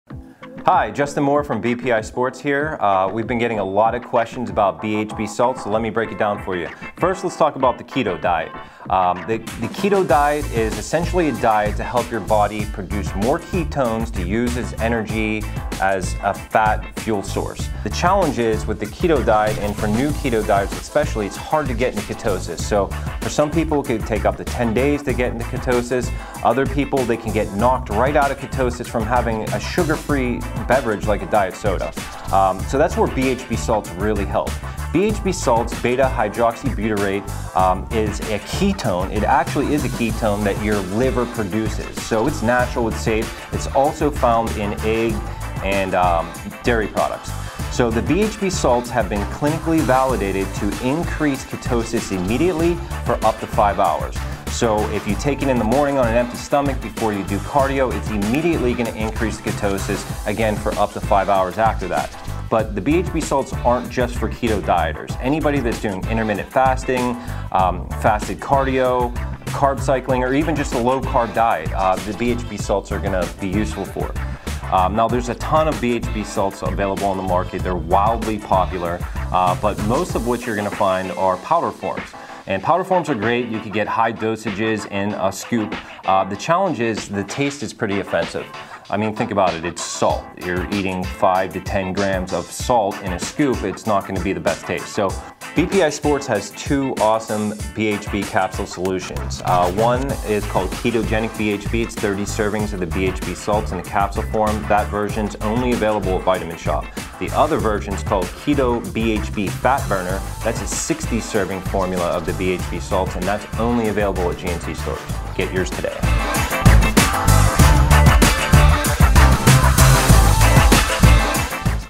What are BHB Salts? - BPI Sports - Keto Diet 101
BHB stands for beta-hydroxybutyrate. On a KetogenicDiet, this fatty acid is used in place of glucose for energy. To accelerate this process, supplement with additional sources of BHB, like the Keto BHB Fat Burner, available only at your local @gnclivewell.